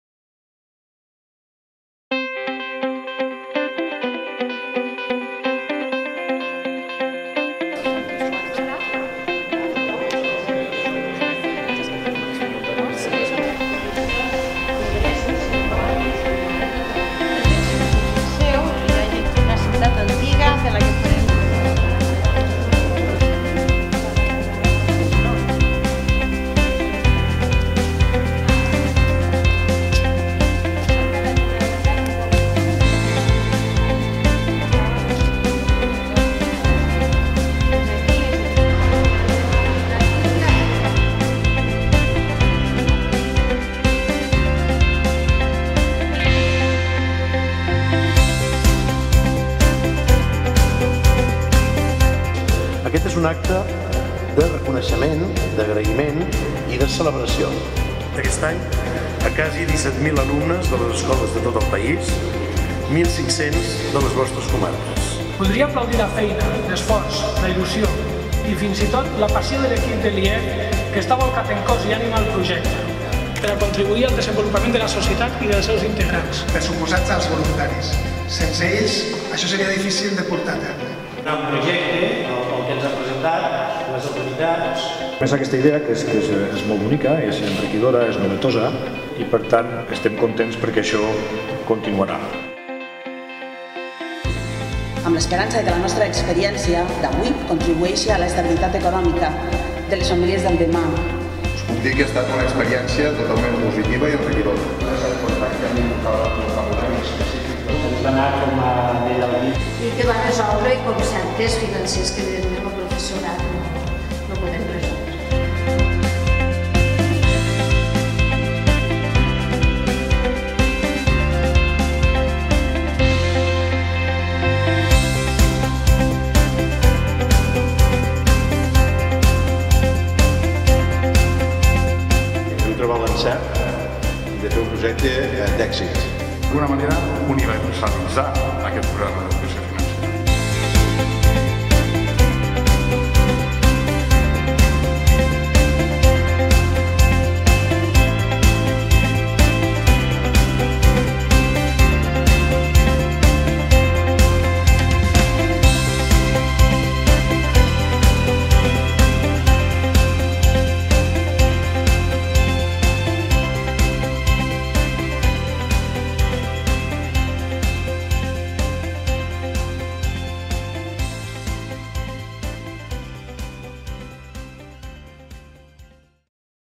IEF - Cloenda Programa EFEC a Lleida
EFEC, una iniciativa pionera
El programa de Educación Financiera en las Escuelas de Cataluña (EFEC) es una iniciativa pionera en el estado español promovida por el partenariado público‐privado formado por los departamentos de enseñanza y Economía de la Generalitat de Catalunya, el Instituto de Estudios Financieros (IEF) y diversas entidades financieras que han actuado tanto como patrocinadores como aportando voluntarios.

Concretamente en la 3ª edición, han participado CaixaBank, Banc Sabadell, BBVA, Banco Santander, Caja de Ingenieros y el Instituto Catalán de Finanzas (ICF). También han aportado voluntarios la Asociación de Usuarios de Bancos, Cajas i Aseguradoras de Cataluña (AICEC‐ADICAE) y la Asociación Española de Asesores iy Planificadores Financieros‐Patrimoniales de España (EFPA España).

En la provincia de Lleida, 50 voluntarios han impartido 282 sesiones en 27 centros escolares para 1.494 alumnos de 4º ESO.